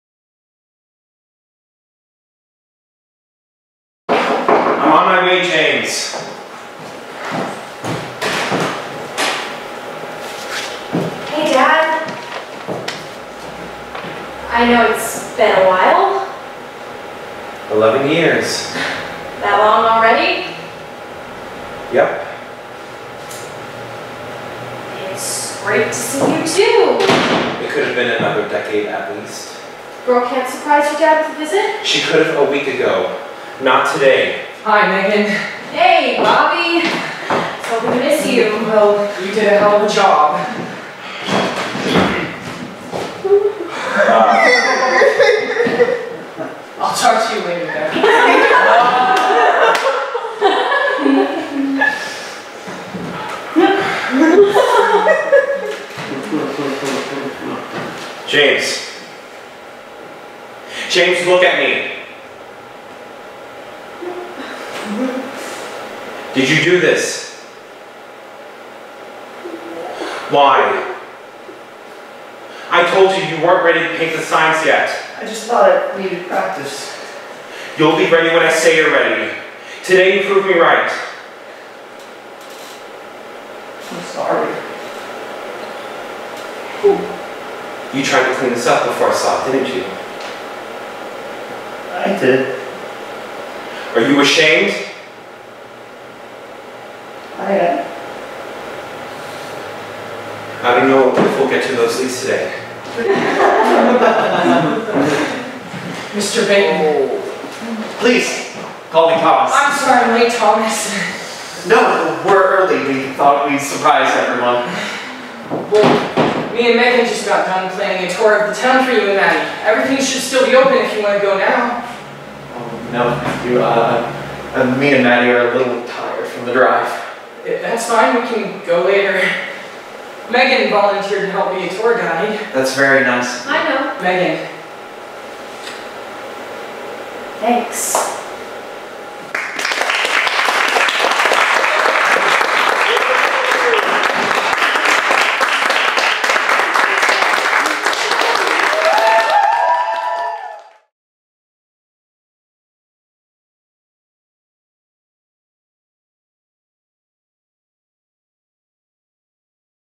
Excerpts from The Sign Maker's Daughter
This video contains excerpts from the 2017 production of The Sign Maker's Daughter.

Written by Monte D. Monteleagre
Directed by Kevin Snyder

Robert Thist- Jonathon Algeroy
Robert "Bobby" Thist Jr.- Noah Tierney
Meagan Baitman- Catie Auchter
James Pachman- Joseph O'Connor
Thomas Baitman- Trevor Harr
Maddie Baitman- Lizzy Fleissner

Stage Manager- Lily L. Craig
Tombstone Designer- Madeline Lacey